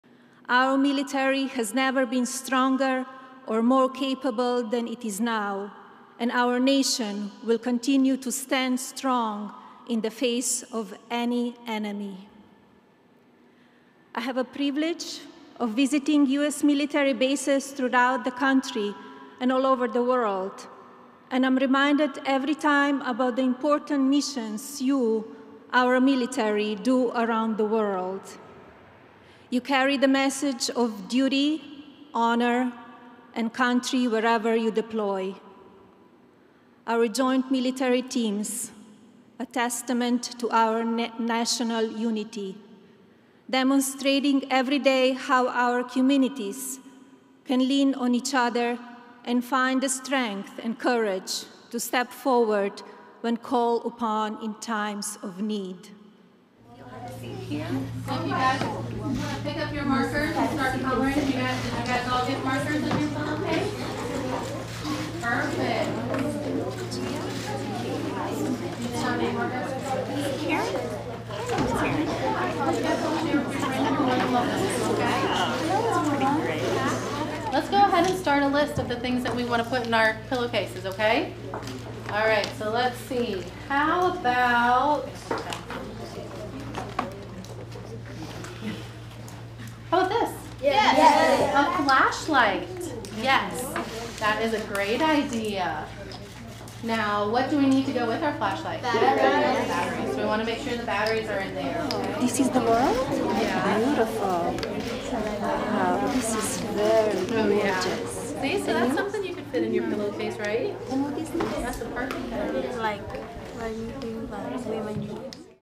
Melania Trump visits with troops, students in S.C.
First Lady Melania Trump and Second Lady Karen Pence traveled to South Carolina Wednesday to talk to both military personnel and students about the state's emergency preparedness and disaster response efforts. (Oct. 30)